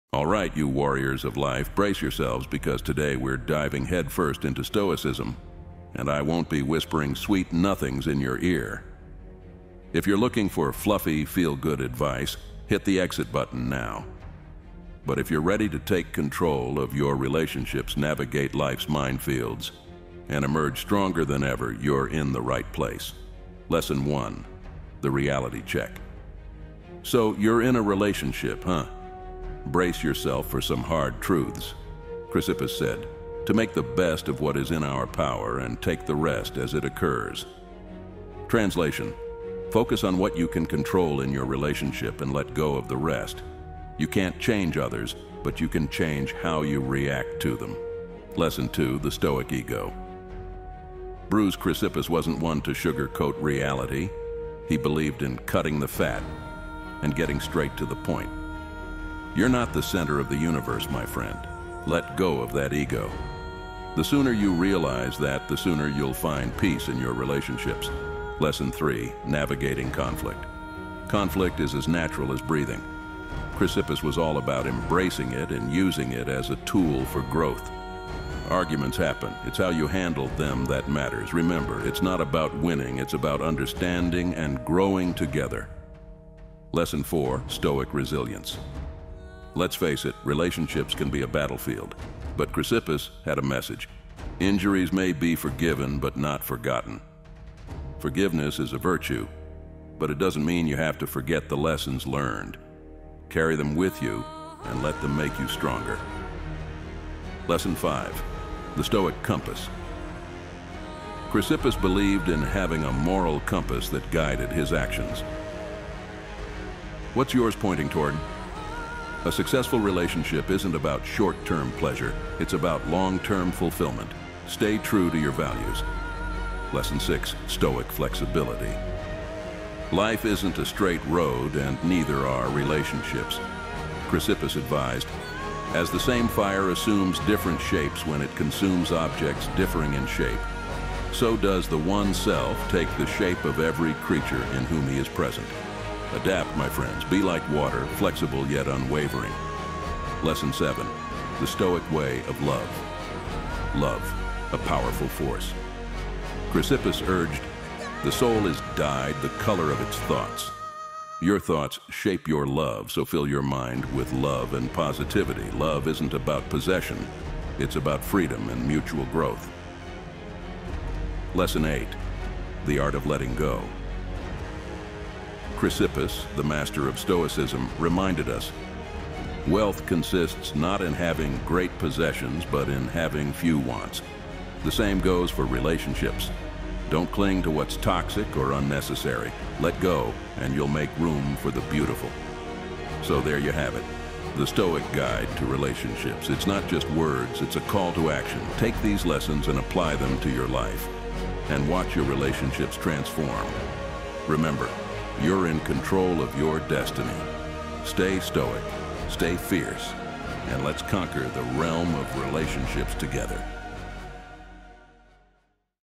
STOIC RELATIONSHIP WISDOM: How to Conquer Life's Storms with Chrysippus
In this powerful video, we're not offering sugar-coated relationship advice. Instead, we dive deep into the profound teachings of Chrysippus, a Stoic philosopher known for his unapologetically realistic insights into life and relationships.

If you're ready to take control of your relationships, navigate the stormy seas of love and conflict, and emerge stronger and wiser, you're in the right place.

Here's a glimpse of what you'll discover:

🔥 Lesson 1: The Reality Check - Focus on what you can control in your relationships and let go of the rest.
🔥 Lesson 2: The Stoic Ego Bruise - Say goodbye to that ego and find peace.
🔥 Lesson 3: Navigating Conflict - Learn how to handle arguments for growth.
🔥 Lesson 4: Stoic Resilience - Understand the power of forgiveness without forgetting.
🔥 Lesson 5: The Stoic Compass - Discover the moral values that guide successful relationships.
🔥 Lesson 6: Stoic Flexibility - Adapt like water, flexible yet unwavering.
🔥 Lesson 7: The Stoic Way of Love - Understand the true nature of love.
🔥 Lesson 8: The Art of Letting Go - Make room for the beautiful by letting go of the toxic.

These Stoic lessons are not just words; they're a call to action. Take them, apply them to your life, and watch your relationships transform. You're in control of your destiny.

Stay Stoic, stay fierce, and let's master the art of relationships together!"

This description provides viewers with a clear overview of the video's content and what they can expect to learn and explore in the video.